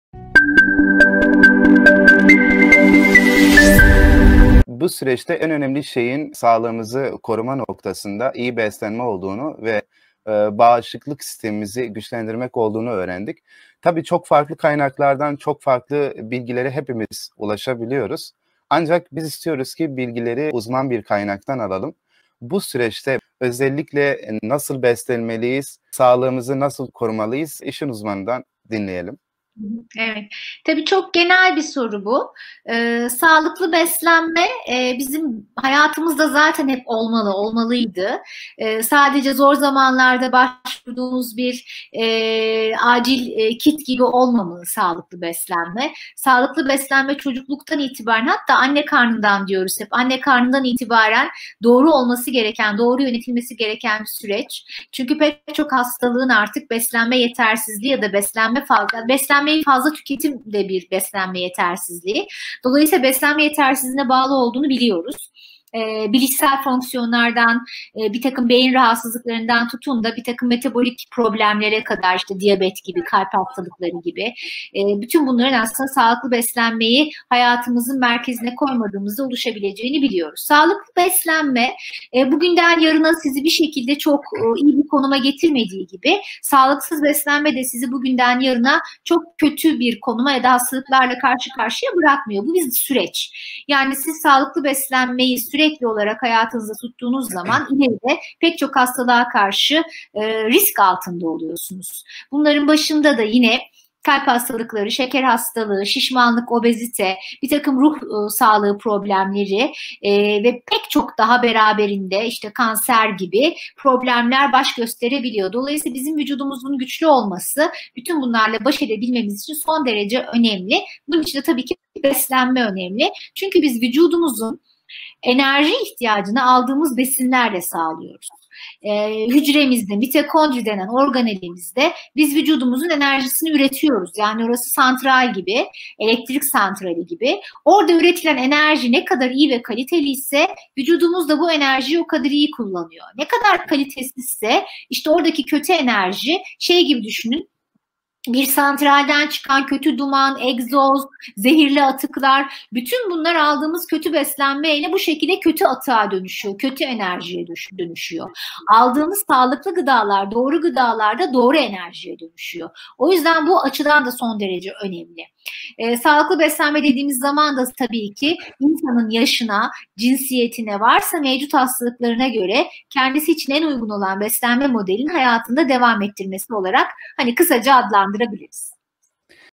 Sağlıklı Beslenme Nedir ?
Uzman Diyetisyen Banu Salman hocamızdan sağlıklı beslenmeyi dinleyelim, sağlıklı beslenme neden önemlidir, bağışıklık sistemine etkileri nelerdir.